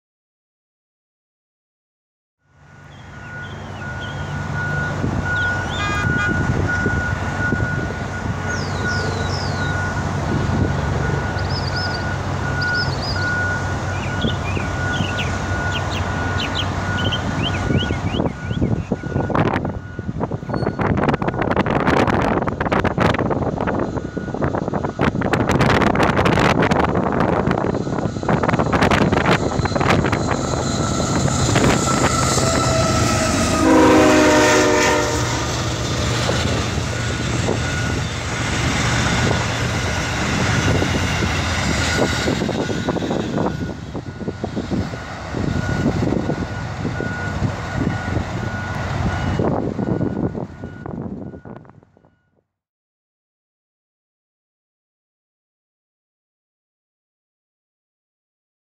VERY RARE: Amtrak P092-20 led by...
I missed the trip to Miami yesterday because of Father's Day but I made it a point not to miss the return trip to New York. Amtrak P092-20 is being led by a CSX ES44AH. This is super rare to see especially down here in South Florida. I shot this video down in Hollywood, FL on the SFRTA Corridor. Sorry about the shakey video as I forgot my tripod at home.  I hope yall enjoy!